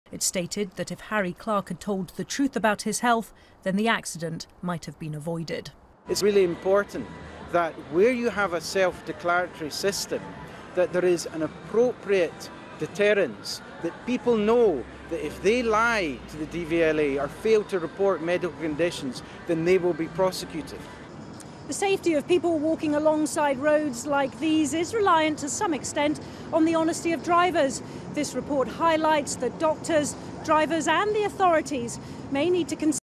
Glasgow bin lorry driver repeatedly lied P4
An inquiry into the Glasgow bin lorry crash has found the tragedy could have been avoided if the driver had not lied about his history of blackouts.
Harry Clarke, 58, was unconscious when the lorry veered out of control on 22 December 2014, killing six people.
The inquiry report states that he "repeatedly lied in order to gain and retain jobs and licences".